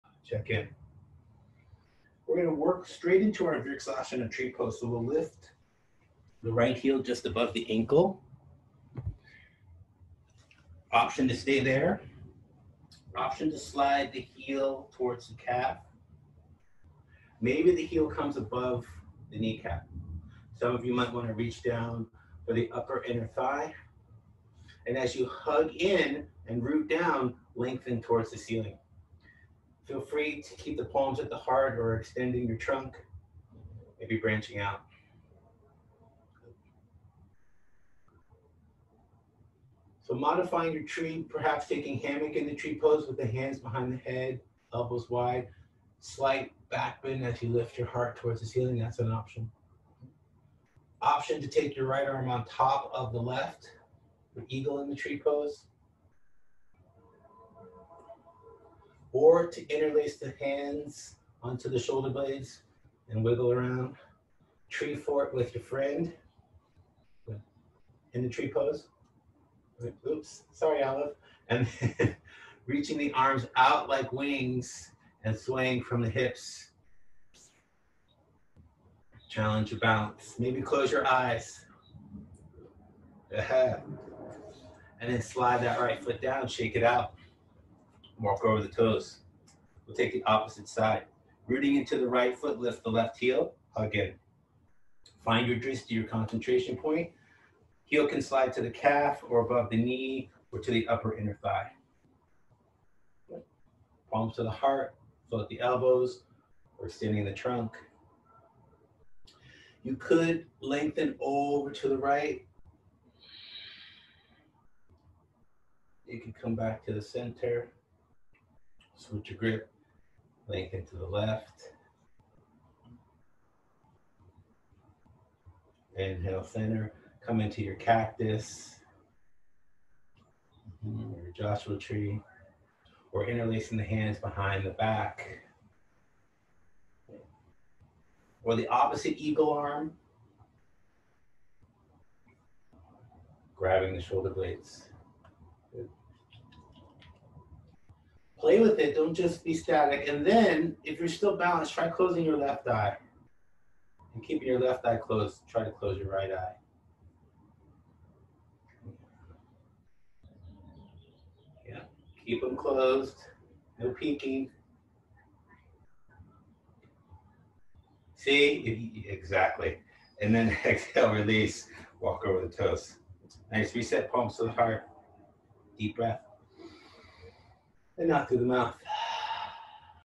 Variations on Tree Pose, with Michael Caldwell
Want to change up your vrksasana (tree pose) practice? Try some of these challenging variations from Michael Caldwell, including: Hammock in the Tree, Eagle in the Tree, Tree Fort with your Friend, Tree pose with side bends, Cactus Tree, Joshua Tree, and options to close your eyes and find movement.

Now offering daily live online yoga classes, visit us @ for a schedule.